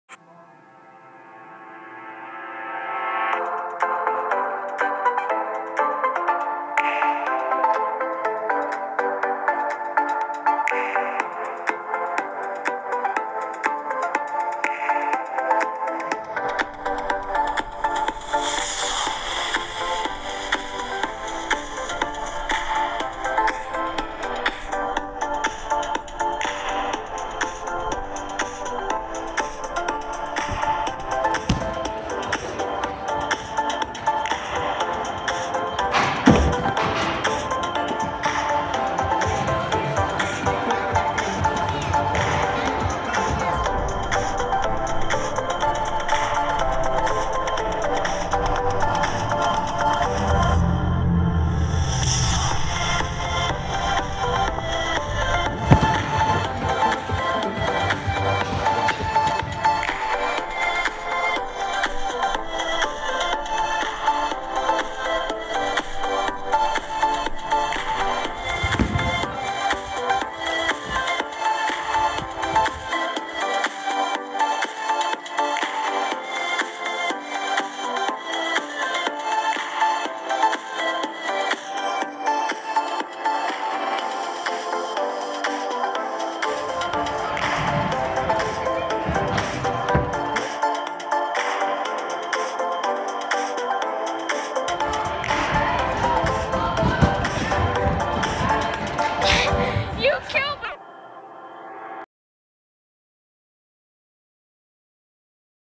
Backflip Progression
Hey Guys! I hope you really enjoyed this video, I am so stoked to have finally landed my standing backflip and can't wait to continue in the future! I also apologize for not doing a LEGO video but I am a bit under the weather and have been drowned in Homework.